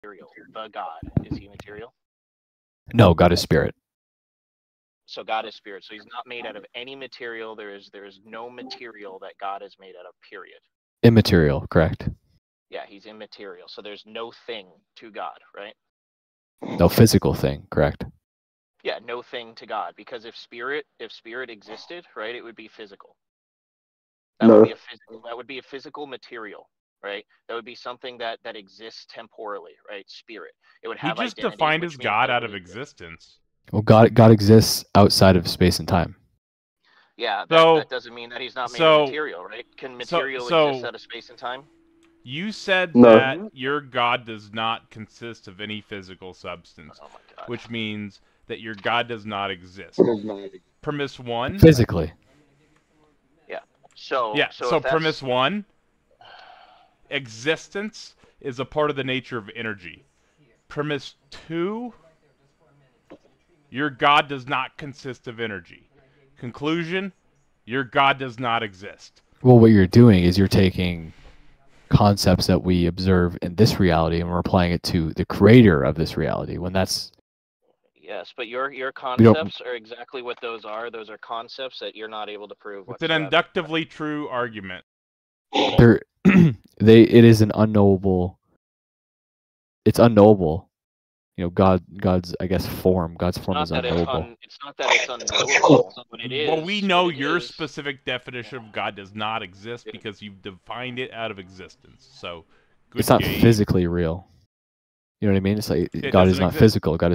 Christian Apologist Defines God Out of Existence
Goose (Christian) takes the time to define his god out of existence.
for more great conversations!
Greg Bahnsen, Cornelius Van Til, Presuppositionalist, Presuppositionalism, Presuppositional Apologetics